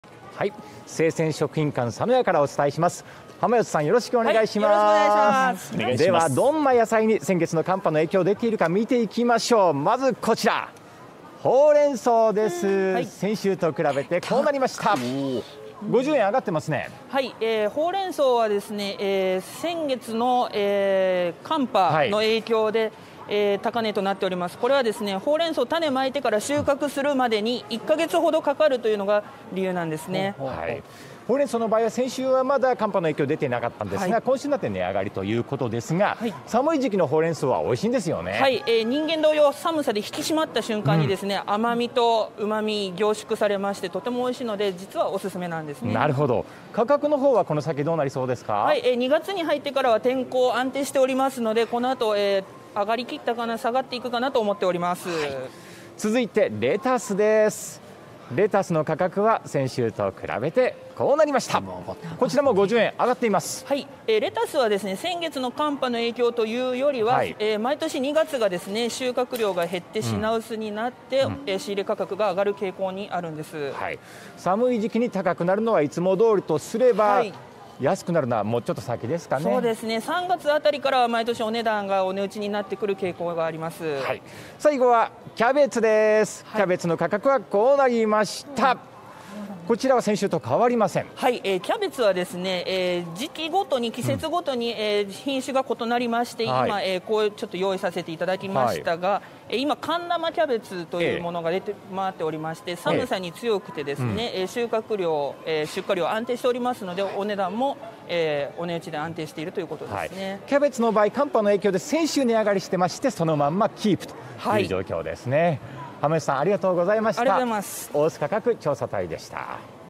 【価格調査】ホウレンソウの価格が上昇 先月の大寒波の影響で生育が遅れ出荷量が減少？ (2023年2月8日)
「大須価格調査隊」です。
野菜の価格は１月の寒波の影響がまだ続いているようです。